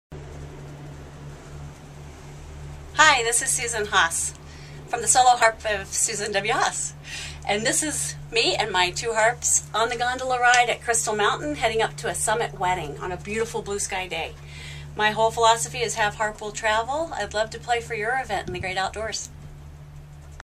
Gondola Ride up Crystal Mountain with two harps
Crystal Mountain Wedding Ceremony, The Solo Harp of Susan Williams Haas, wedding harpist in Seattle/Tacoma, riding the Gondola to the top of Crystal to play for a Summit wedding ceremony July 16, 2011